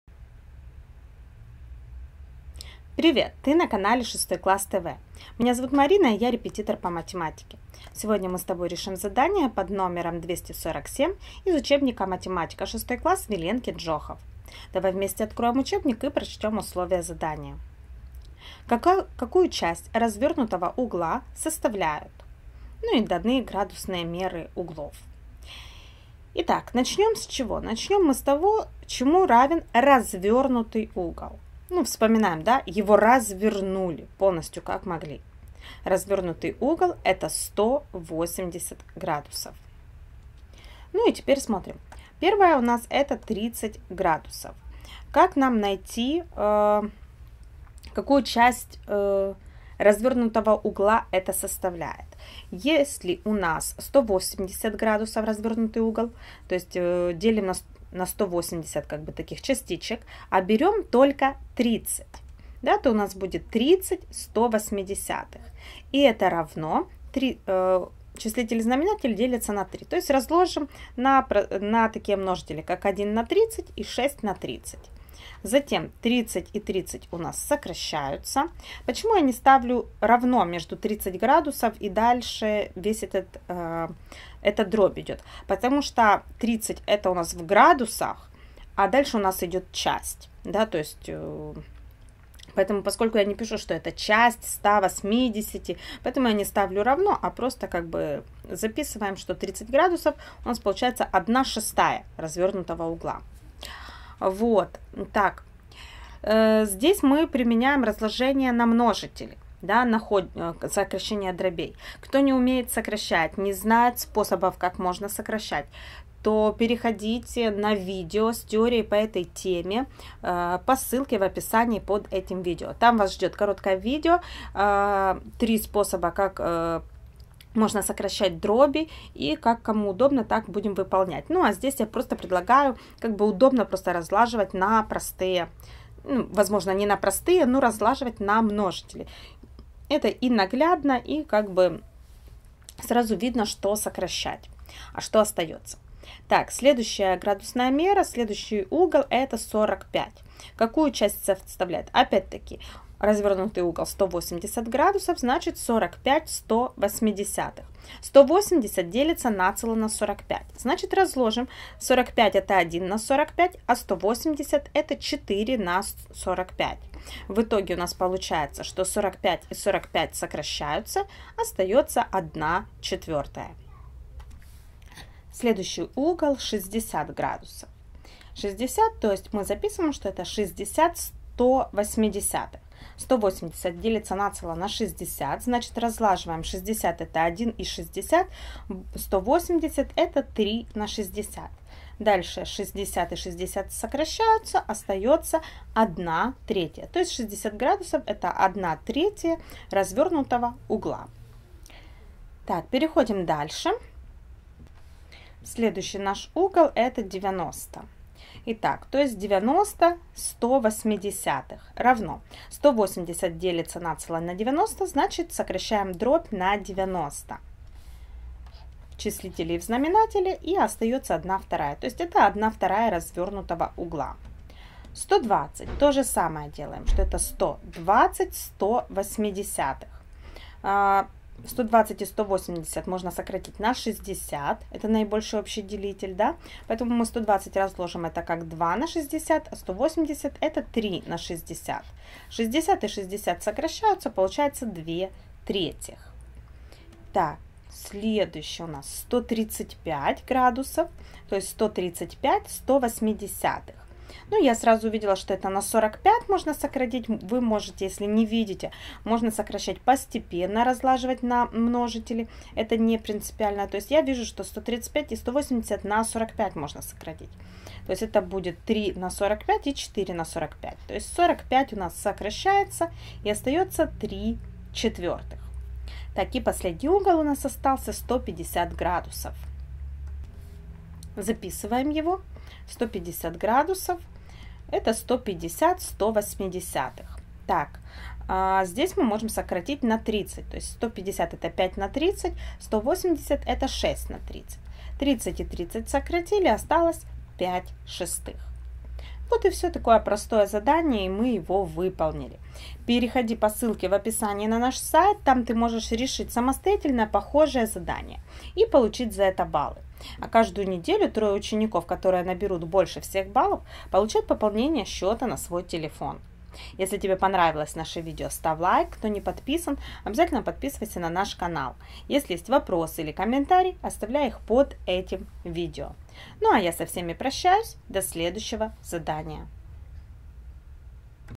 Задание №247 - ГДЗ по математике 6 класс (Виленкин)
Для того чтобы выполнить задание №247, в котором требуется найти часть угла, представленную в градусах, нужно уметь сокращать дроби.
Это задание не займет у вас много времени, а если вы воспользуетесь нашим видео-решением, то вам хватит 6-ти минут вашего времени.